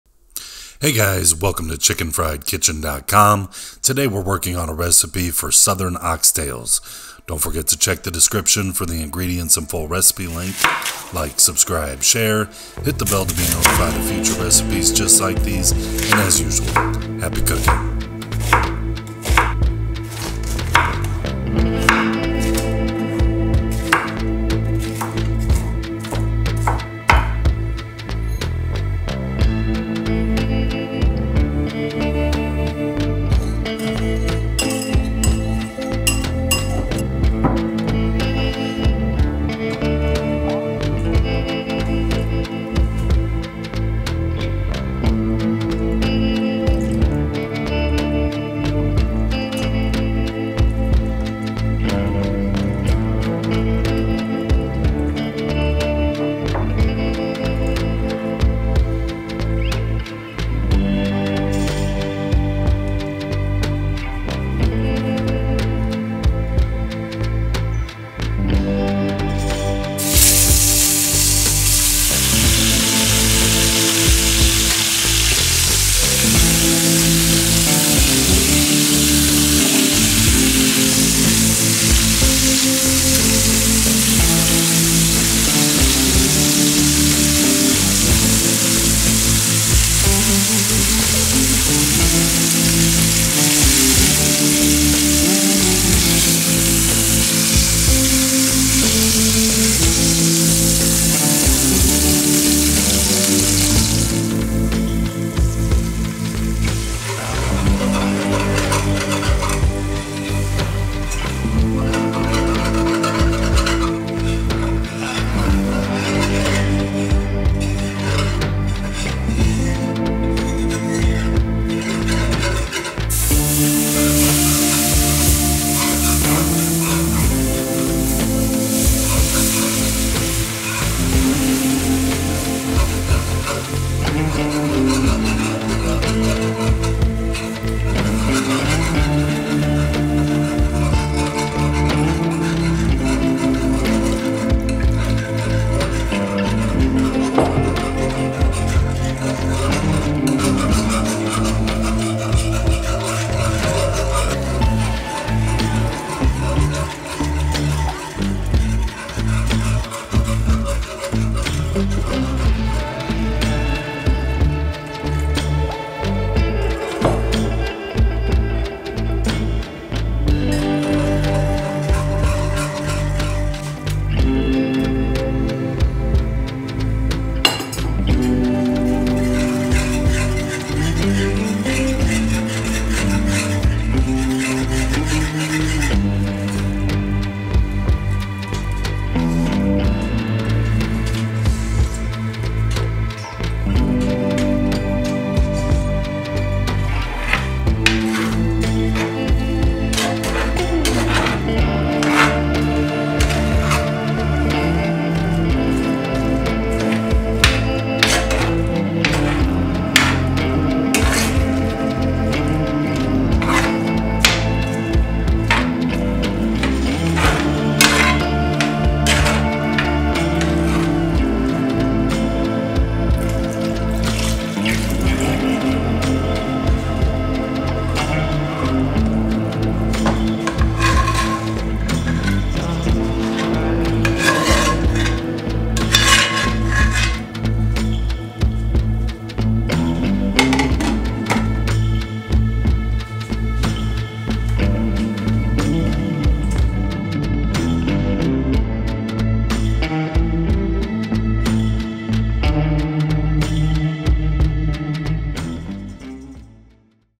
Slow Cooker Southern Oxtails Recipe
Hey guys! Welcome to Chicken Fried Kitchen. Today, we're working on my delicious and easy-to-follow Southern Oxtails Recipe. If you're a fan of soul food or just looking for a comforting, hearty meal, this dish is for you! Slow-cooked to perfection, these oxtails are tender, flavorful, and smothered in a rich, savory gravy that's perfect over rice or mashed potatoes. Whether you’re cooking for family, friends, or yourself, this recipe will definitely become a go-to.

Ingredients:
2 Tablespoons Kosher Salt
1 ½ Tablespoons ground Black Pepper
1 teaspoon White Pepper
1 teaspoon Italian Seasoning
4 lbs Oxtails
4 Tablespoons Worcestershire Sauce
4 Tablespoon Browning
4 Tablespoons Vegetable Oil
4 Cups Beef Stock Divided
1 Yellow Onion Sliced
1 Green Bell Pepper Chopped
2 tablespoons Fresh Minced Garlic
2 Bay Leaves